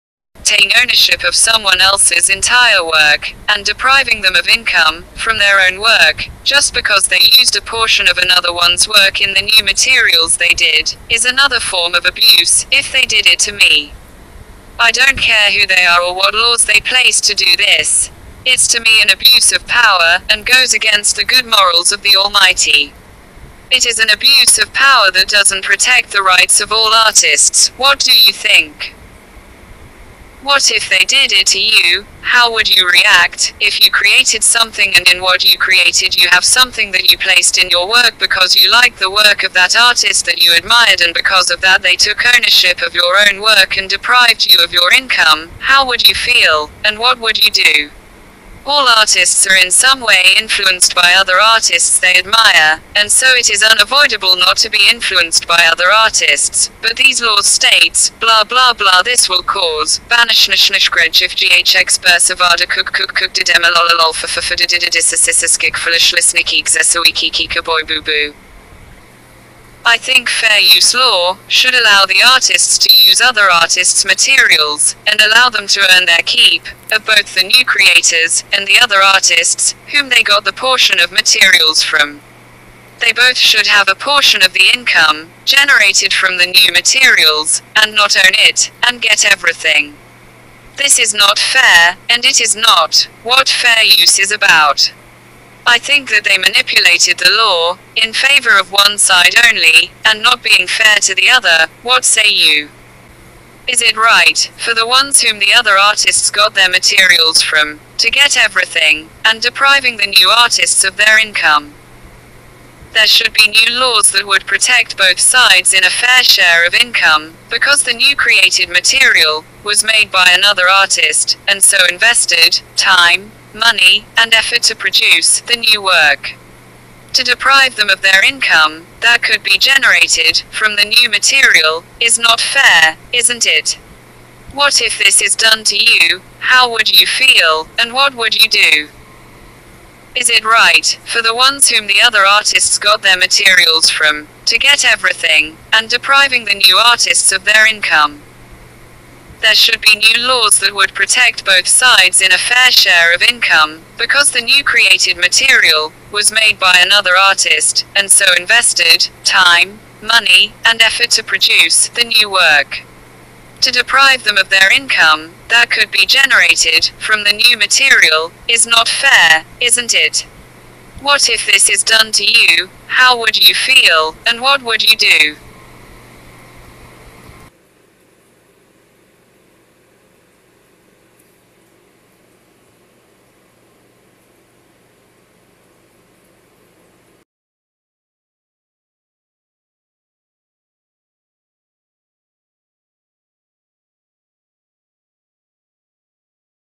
The Artist and Fair Use Law
I think that taking ownership of someone else's entire work just because they used a portion of another ones work in the new materials they did is an abuse if they did it to me I don't care who they are or what laws they placed to do this is to me an abuse of power and goes against the Good morals of the ALMIGHTY.. It is an Abuse of power that doesn't protect the rights of all artists, what do you think?